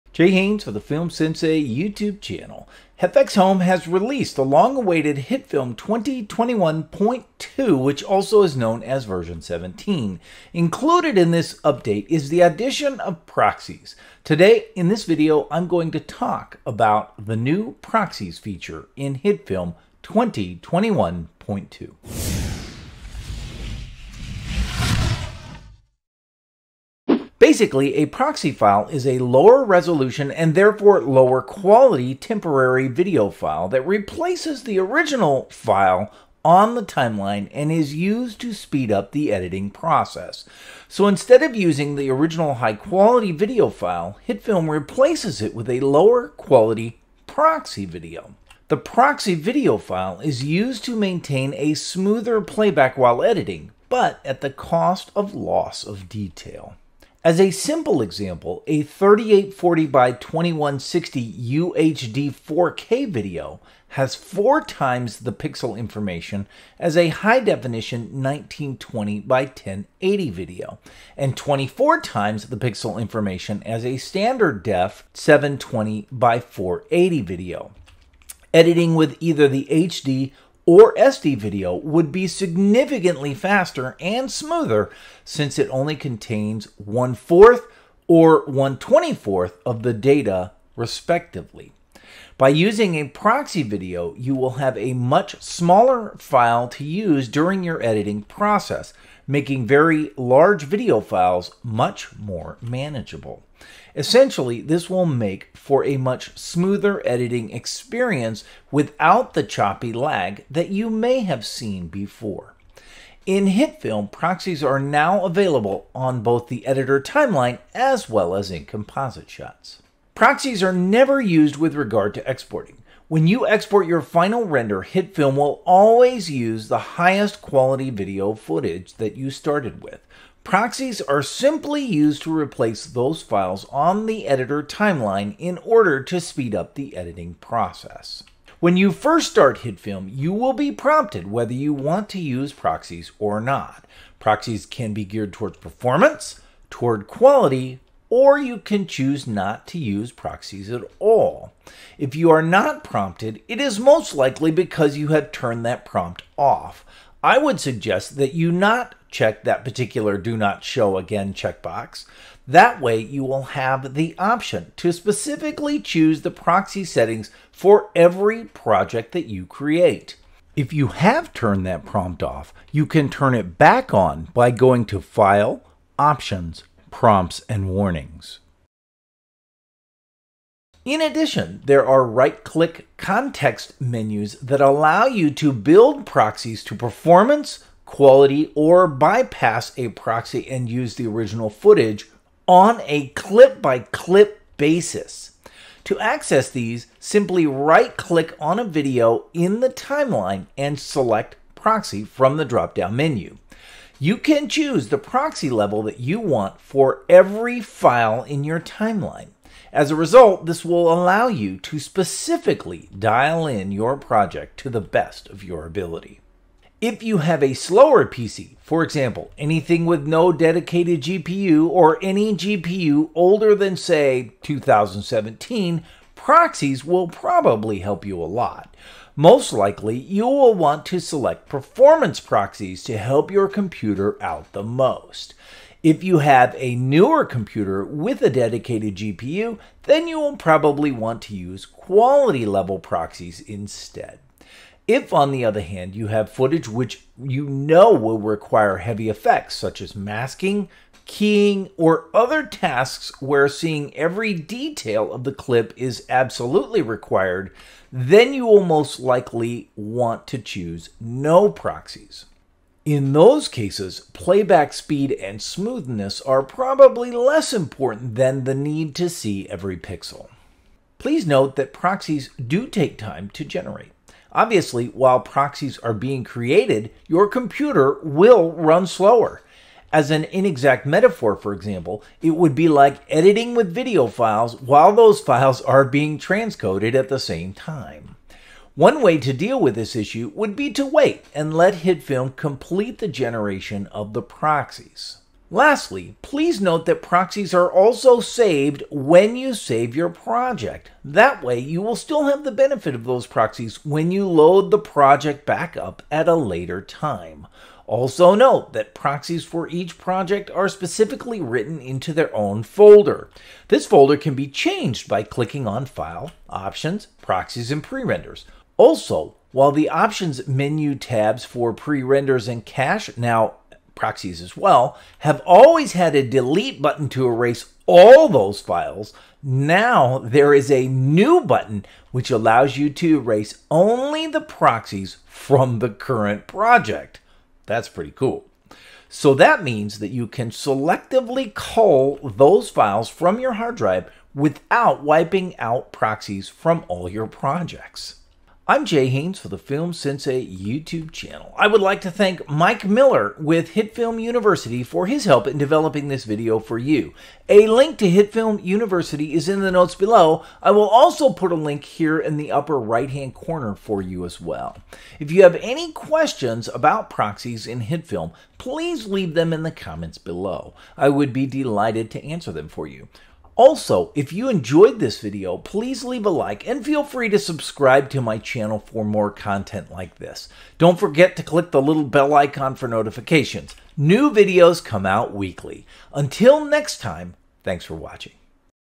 Proxies in HitFilm 2021.2 (Version 17)
Do you want to learn everything there is to know about proxies in HitFilm 2021.2 (Version 17)? Well look no further! In this video, I talk about everything regards proxies in HitFilm 2021.2 (Version 17).

Table of Contents:
0:00 - Introduction
0:28 - What is a Proxy File?
2:00 - Are Proxies Used in Export?
2:18 - Defining Proxy Levels
3:00 - Proxies on a Clip by Clip Basis
3:36 - Which Proxy Level Should I Choose?
4:31 - Proxies Generate in the Background
4:55 - Proxies Are Saved with the Project
5:48 - Wrap Up!